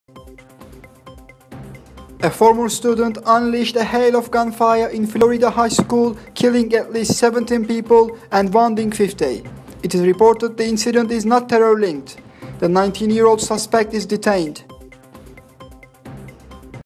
AT LEAST 17 DEAD IN FLORIDA SCHOOL SHOOTING
A former student unleashed a hail of gunfire in a Florida high school killing at least 17 people and wounding 50.  It is reported the incident is not terror linked. The 19-year-old suspect is detained.